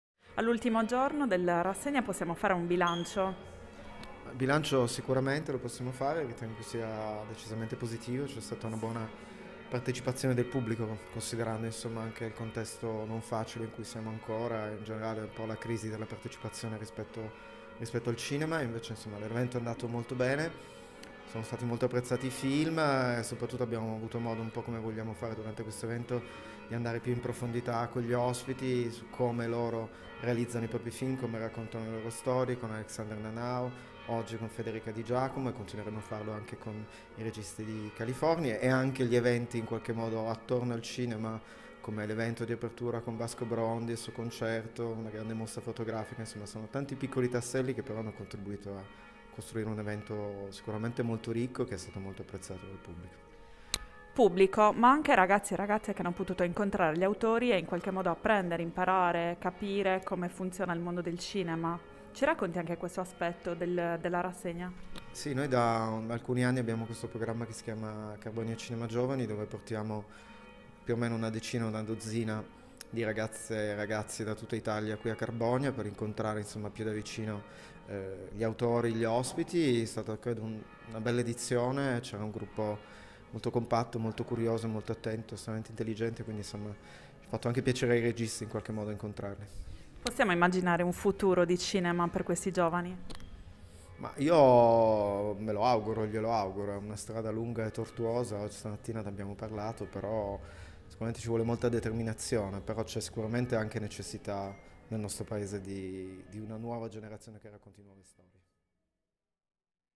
Carbonia Film Festival - How to Film the World, intervista al direttore artistico Francesco Giai Via
Il direttore artistico di Carbonia Film Festival - How to Film the World, rassegna biennale di cinema a Carbonia, traccia un bilancio della terza edizione.